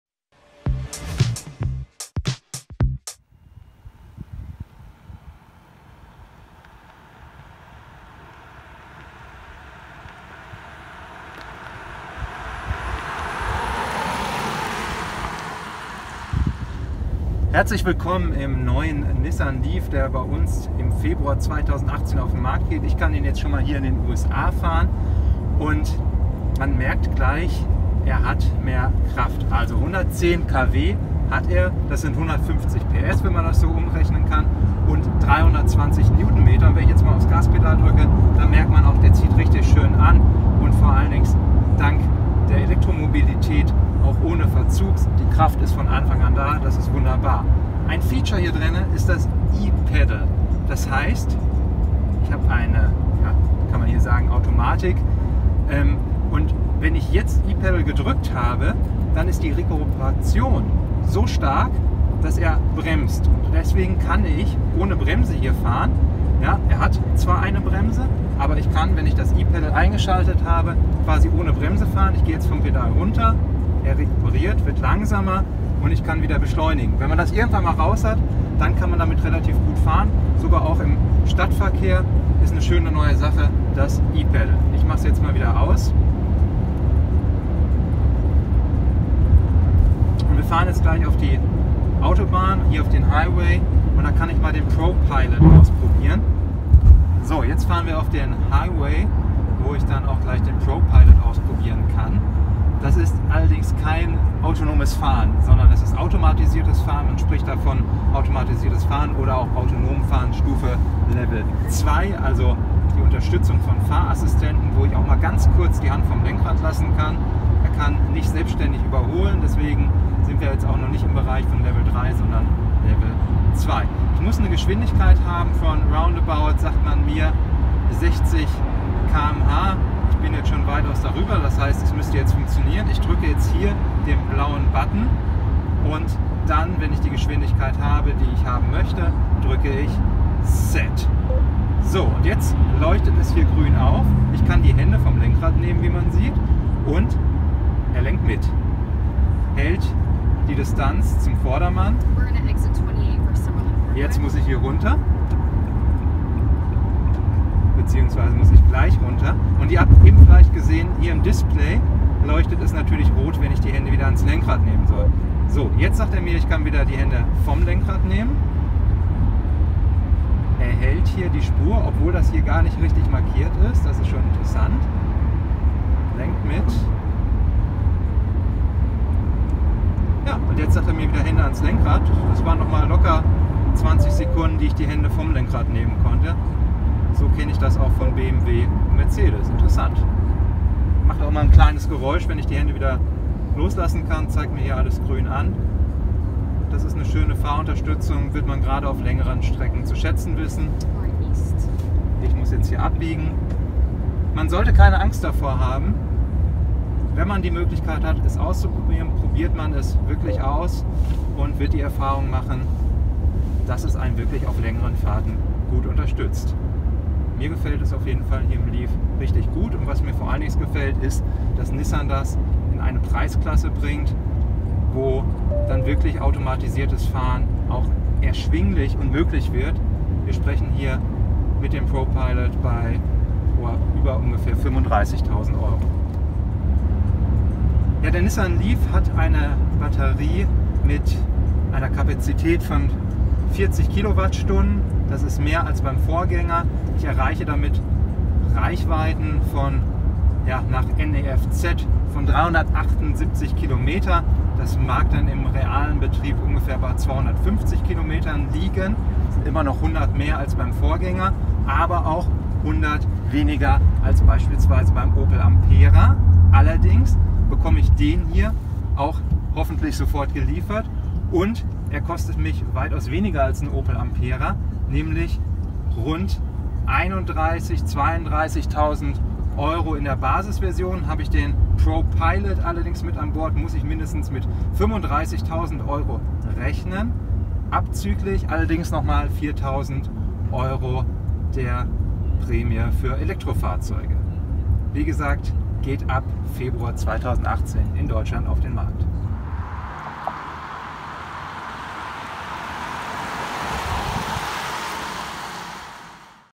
Quickshot Nissan Leaf (2018) Details/Erklärung
Nissans Leaf geht als meistverkauftes E-Auto in die zweite Generation: Der Stromer ist stärker geworden und fährt sogar teilautonom.